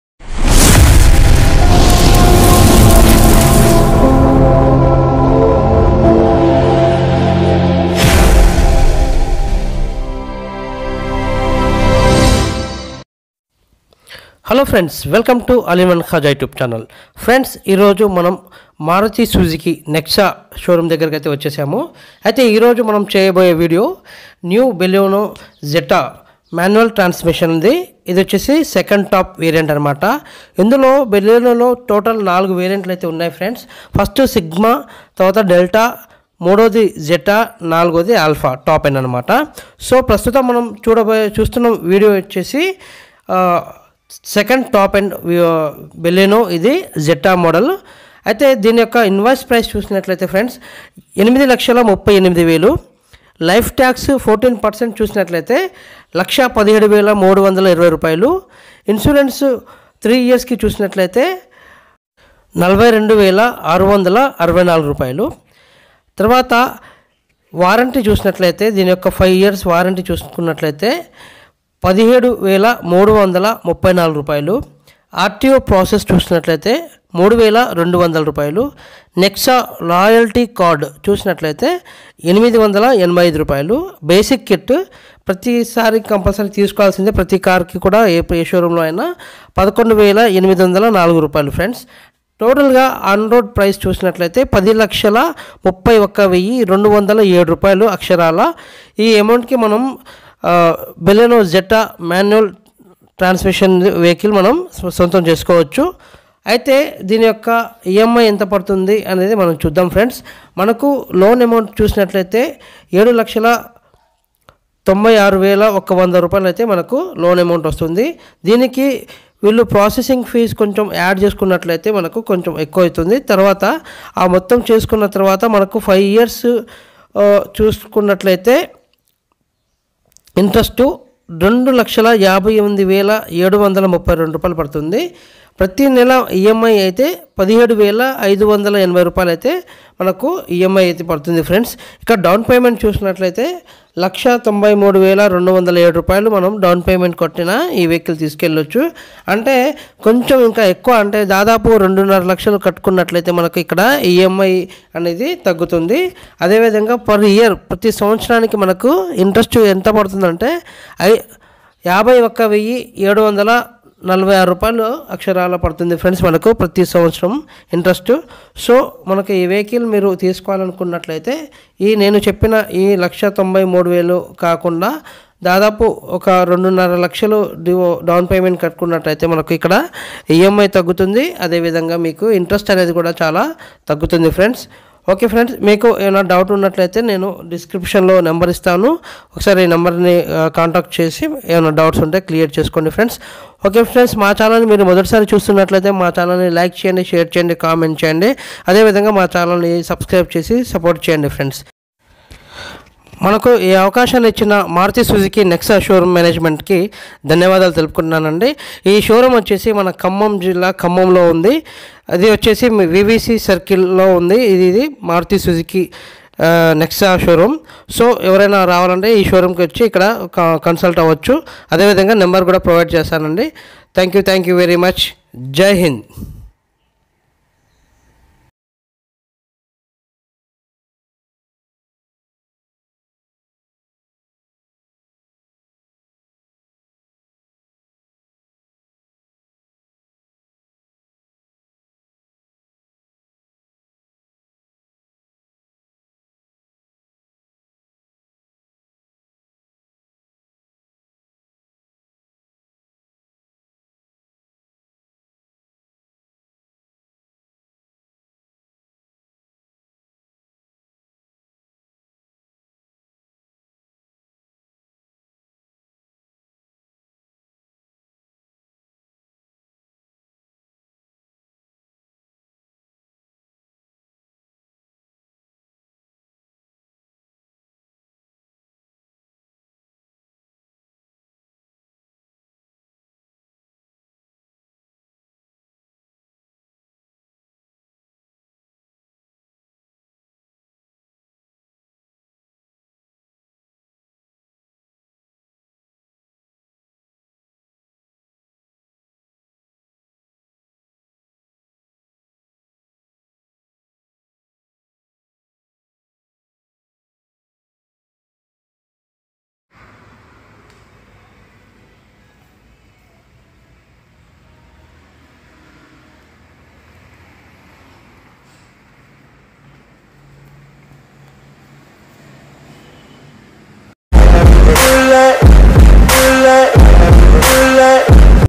NEXA||BALENO||2ndtop||varient||down payment ఎంత కట్టాలి,EMI ఎంత కట్టాలి
BALENO 2nd top varient down payment and EMI ఎంత కట్టాలి...